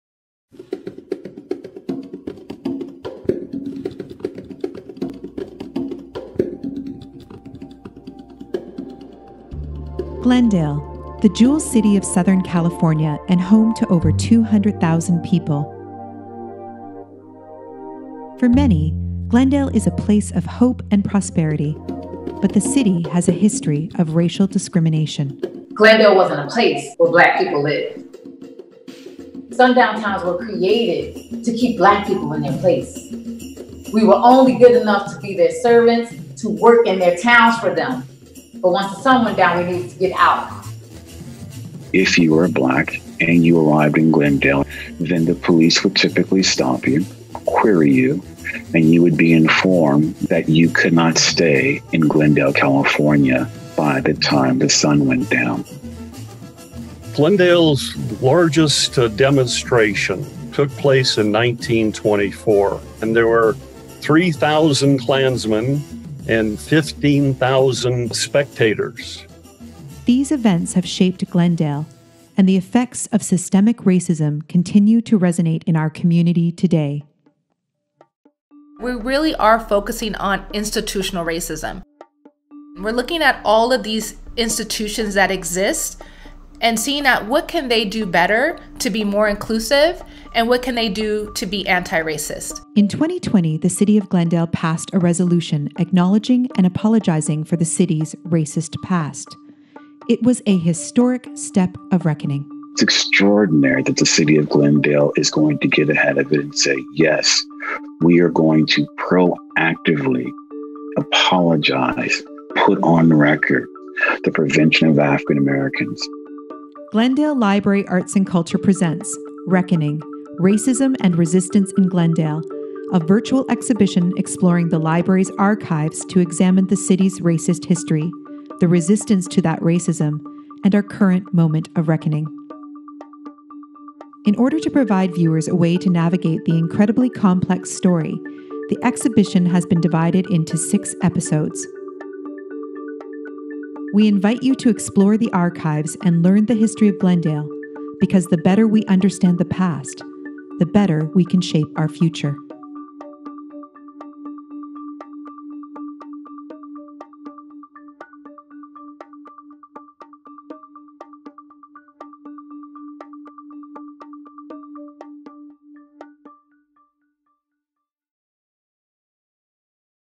2022 CLA PRExcellence Awardee $10+ Million Budget: Electronic (Tie)
Glendale Library Arts and Culture
Reckoning: Racism & Resistance in Glendale